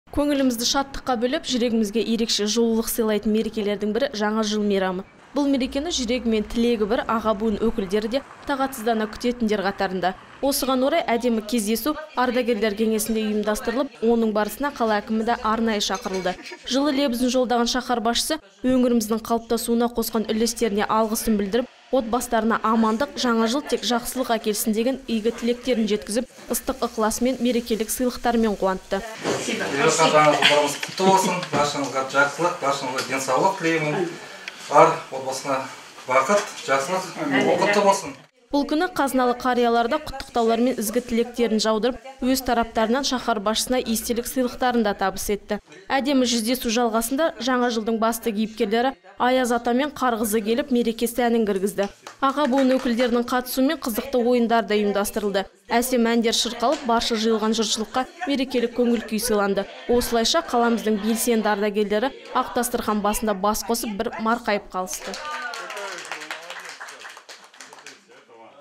Ғажайыпқа толы – жаңа жыл!
Көңілімізді шаттыққа бөлеп, жүрегімізге ерекше жылулық сыйлайтын мерекелердің бірі-Жаңа жыл мейрамы.

Бұл мерекені жүрегі мен тілегі бір аға буын өкілдері де тағатсыздана күтетіндер қатарында. Осыған орай әдемі кездесу ардагерлер кеңесінде ұйымдастырылып, оның барысына қала әкімі де арнайы шақыртылды. Жылы лебізін жолдаған шаһар басшысы өңіріміздің қалыптасуына қосқан үлестеріне алғысын білдіріп, отбастарына амандық , жаңа жыл тек жақсылық әкелсін деген игі тілектерін жеткізіп,ыстық ықыласымен . мерекелік сыйлықтармен қуантты.

Бұл күні қазыналы қариялар да құттықтаулар мен ізгі тілектерін жаудырып, өз тараптарынан шаһар басшысына естелік сыйлықтарын да табыс етті. Әдемі жүздесу жалғасында жаңа жылдың басты кейіпкерлері Аяз Ата мен Қар қызы келіп, мереке сәнін кіргізді. Аға буын өкілдерінің қатысумен қызықты ойындар да ұйымдастырылды. Әсем әндер шырқалып, барша жиылған жұртшылыққа мерекелік көңіл күй сыйланды. Осылайша, қаламыздың белсенді ардагерлері ақ дастархан басында бас қосып, бір марқайып қалысты.